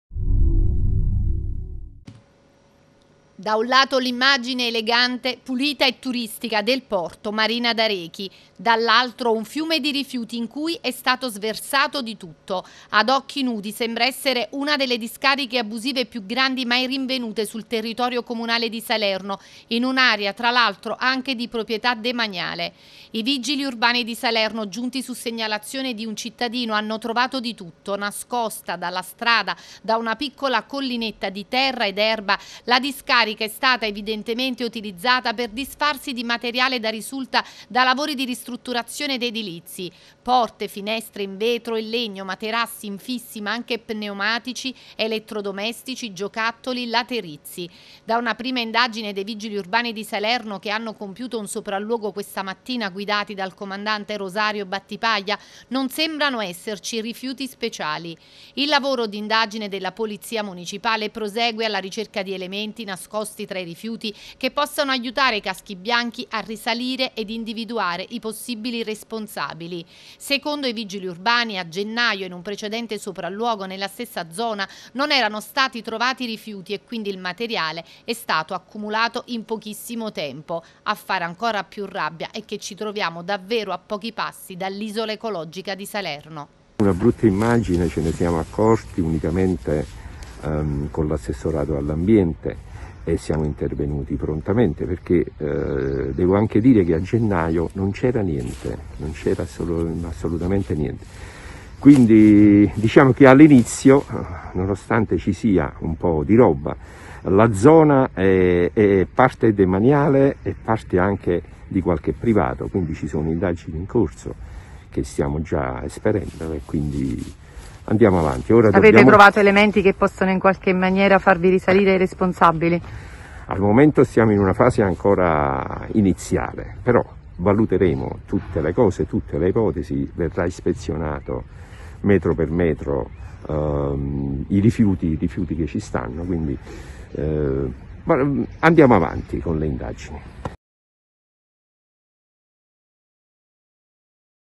Salerno, scoperta discaricaabusiva a pochi passi dal MarinadArechi
Da un lato l’immagine elegante, pulita e turistica del Porto Marina d’Arechi. Dall’altro un fiume di rifiuti, in cui è stato sversato di tutto. Ad occhi nudi, sembra essere una delle discariche abusive più grandi mai rinvenute sul territorio comunale di Salerno, in un’area anche di proprietà demaniale. I vigili urbani di Salerno, giunti su segnalazione di un cittadino hanno trovato di tutto.